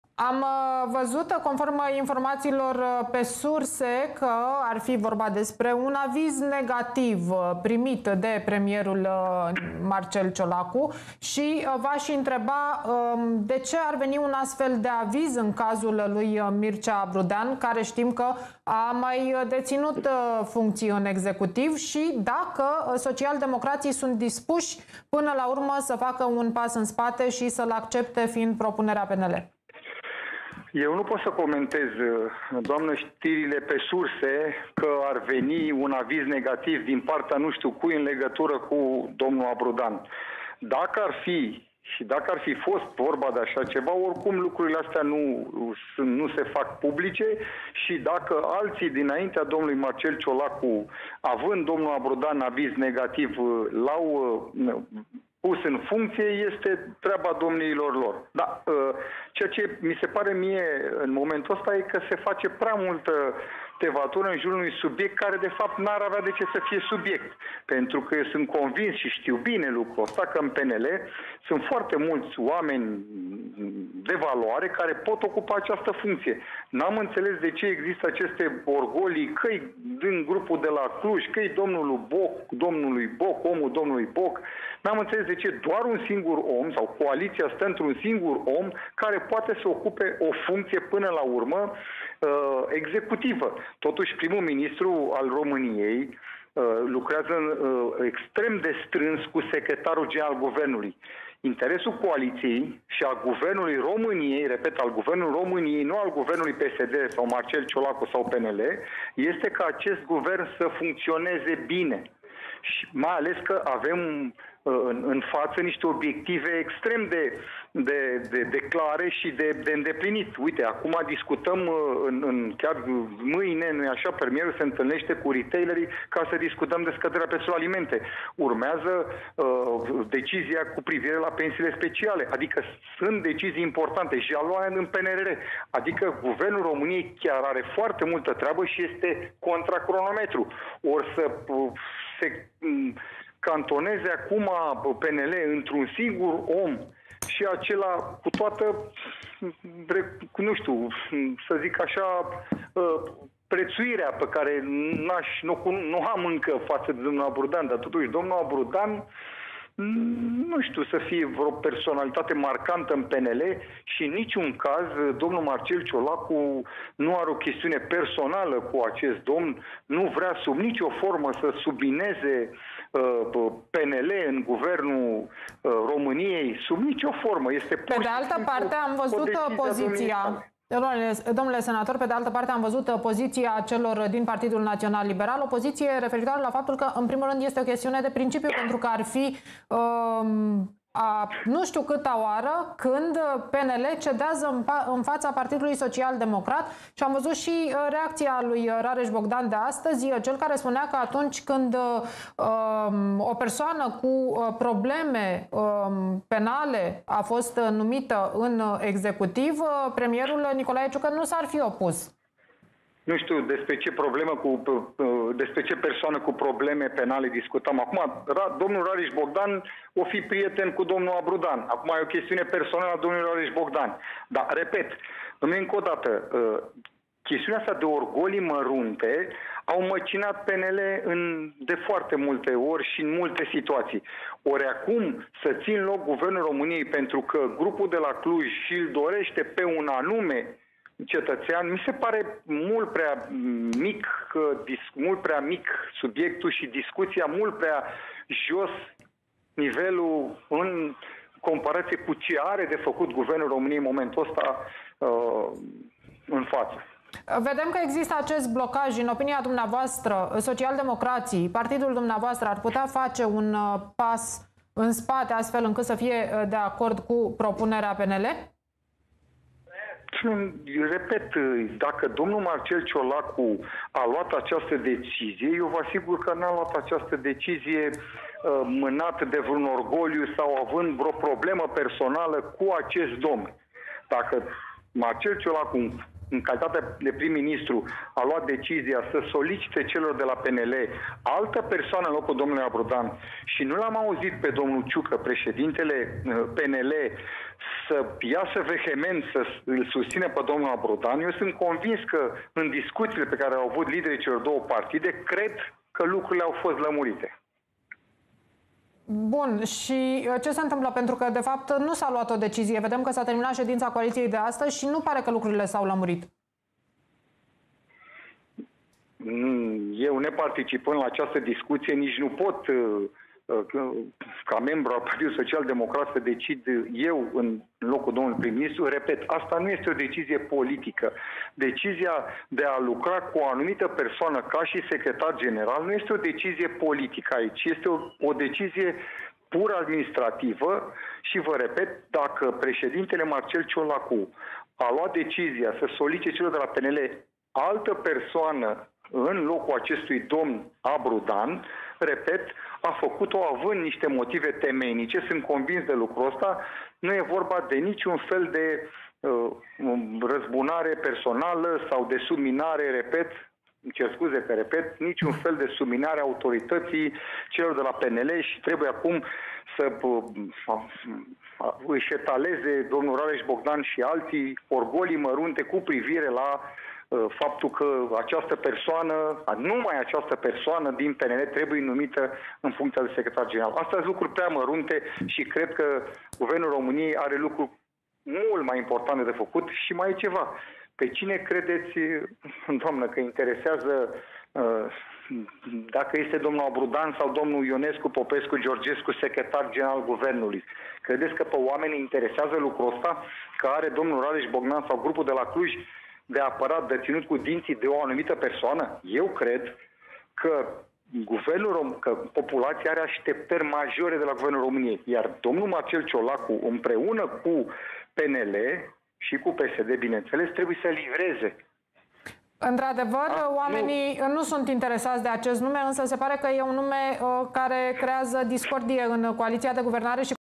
Daniel Zamfir (PSD), după ce premierul Ciolacu l-a respins pe liberalul Abrudean pentru șefia SGG:
” Liberalii să vină cu alt nume. Chestiunea asta de orgolii mărunte a măcinat PNL de multe ori”